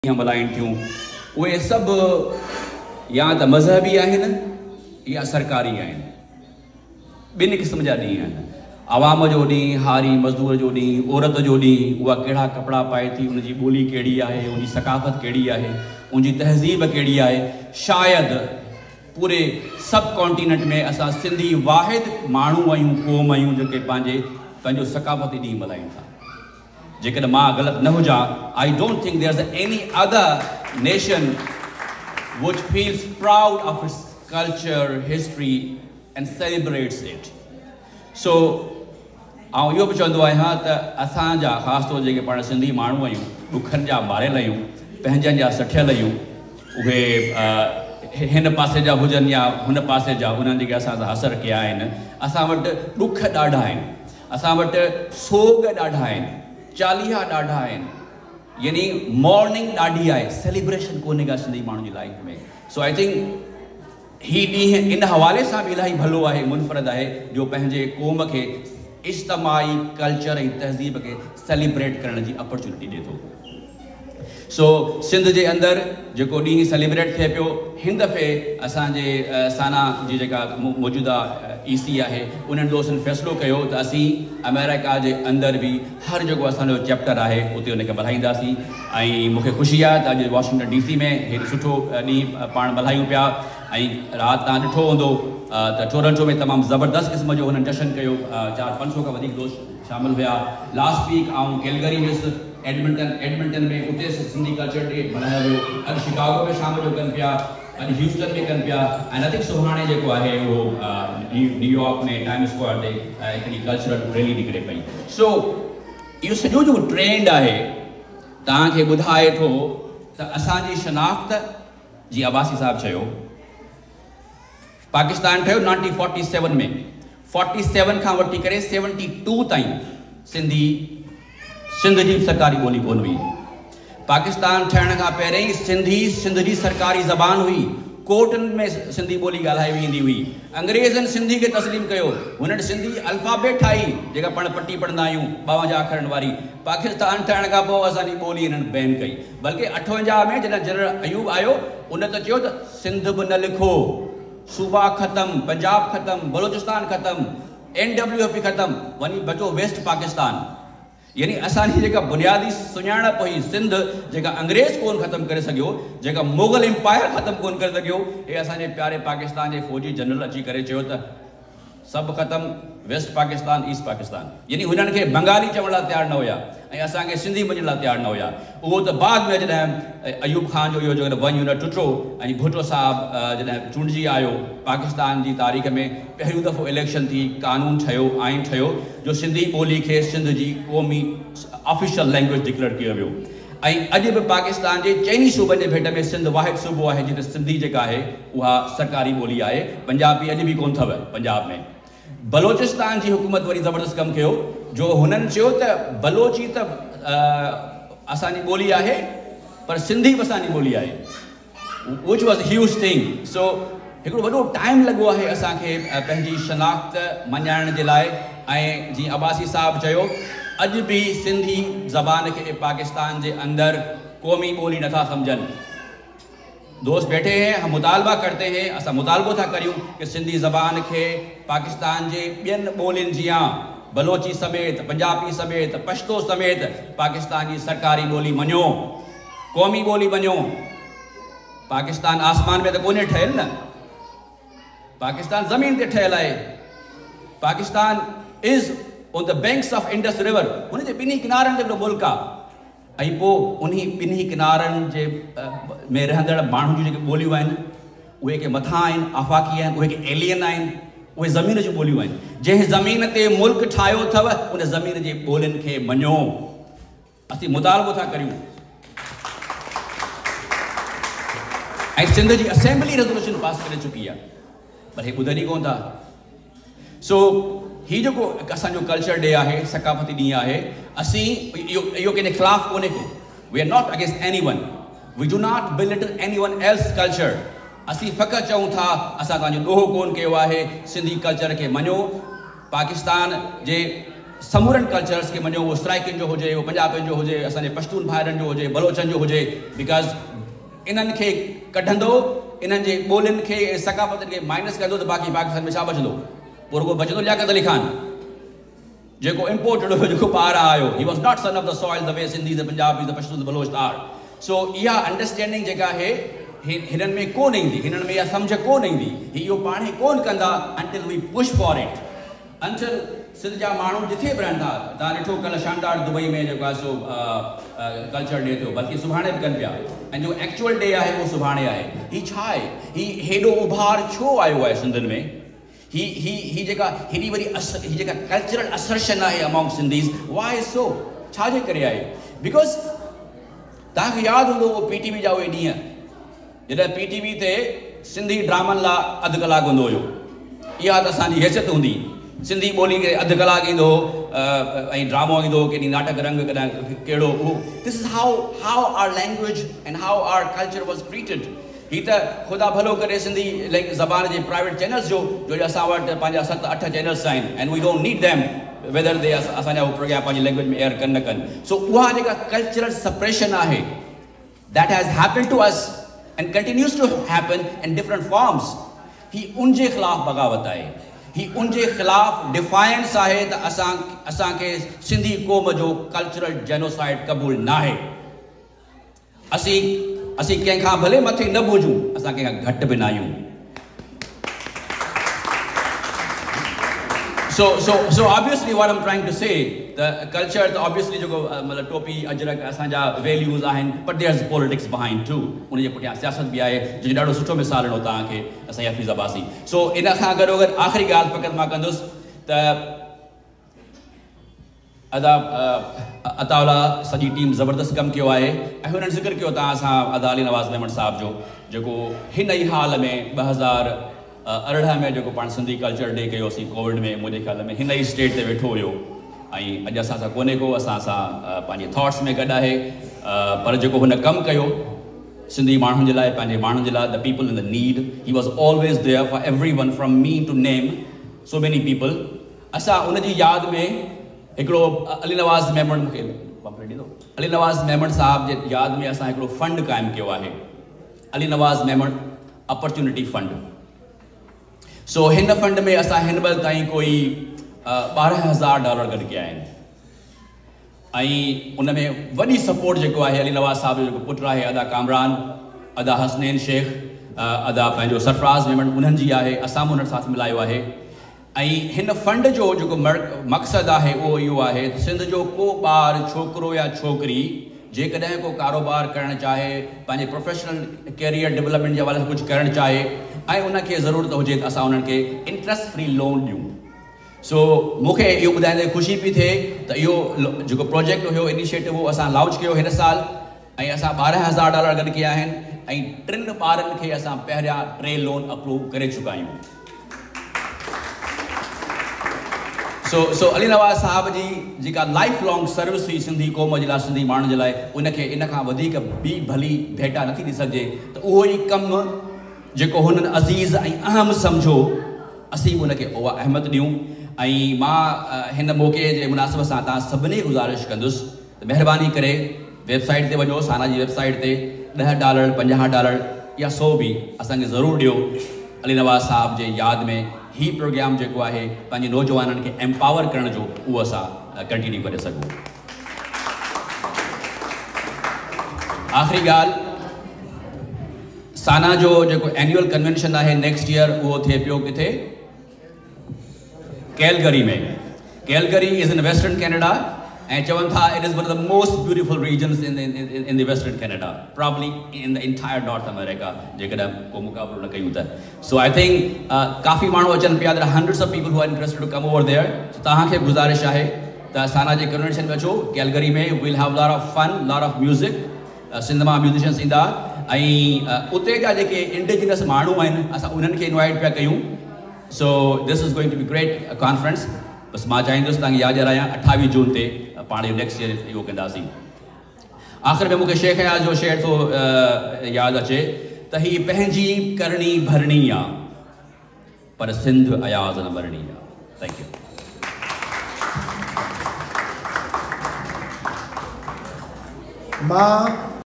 سنڌي ڪلچر ڊي/مشتاق راڄپر/واشنگٽن ڊي سي ۾ تقرير
SANA's General Mushtaq Rajpar explains rise of Sindhi culture as a movement of celebration and cultural assertion, at the SANA Washington DC annual event